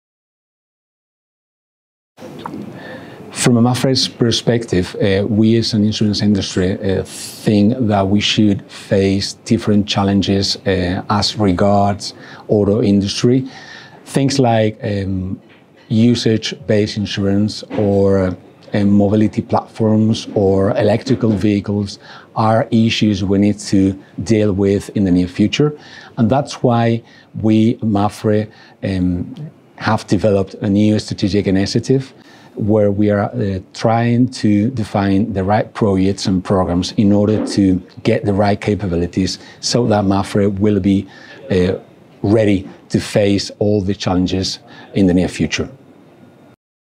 Some of the challenges the insurance sector is facing with automobiles
In this video, José Manuel Corral, Group Chief Business Officer at MAPFRE, tells us about current mobility trends and how MAPFRE is getting ready for what’s coming down the road.